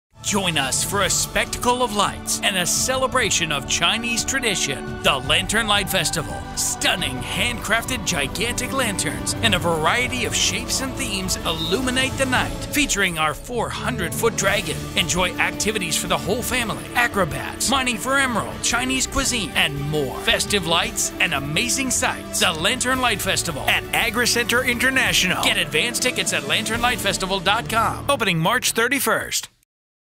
Lantern Light Festival Memphis
Experience an evening of dazzling lights and sights Miami Lantern Light Festival over 60 enormous lanterns, hand-crafted by Chinese artisans. Tickets now on sale for the Memphis Lantern Light Festival. Limited VIP tickets available for each night.